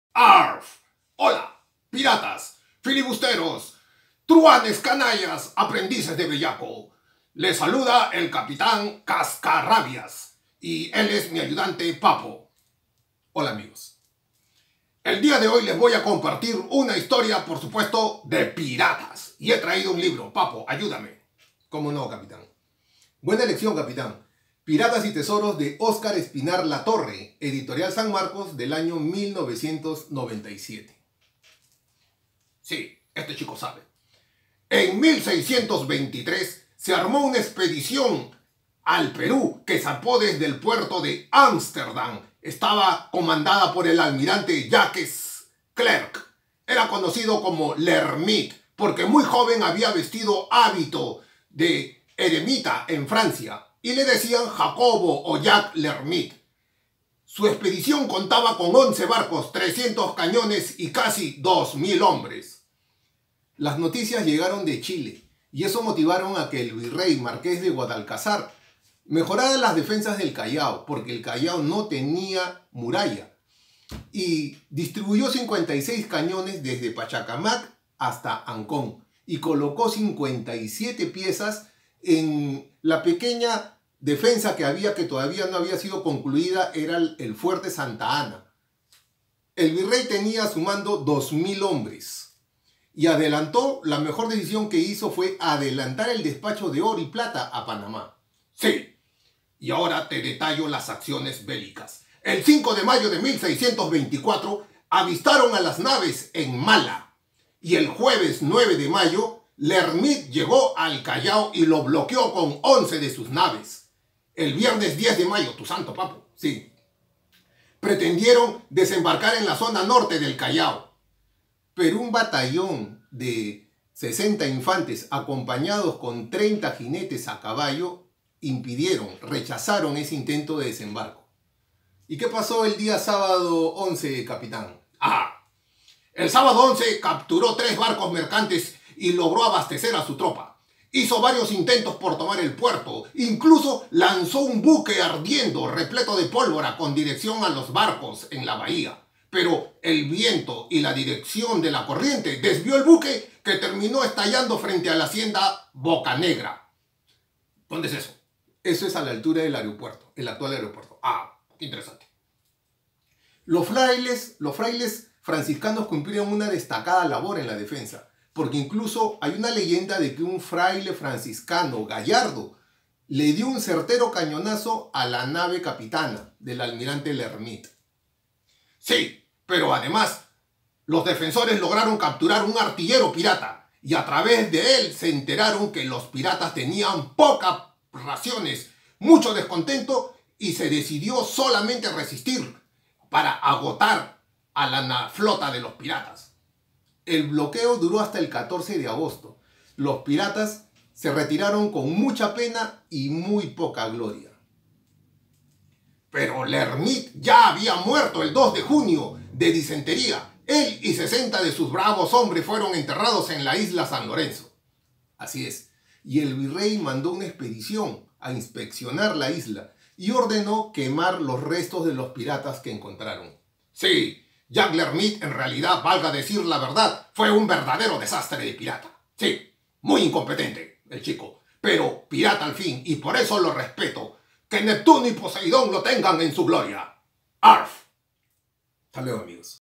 El pirata Jacques l'Hermite ataca el Callao
El pirata holandés Jacques l'Hermite asedia el Callao en mayo de 1624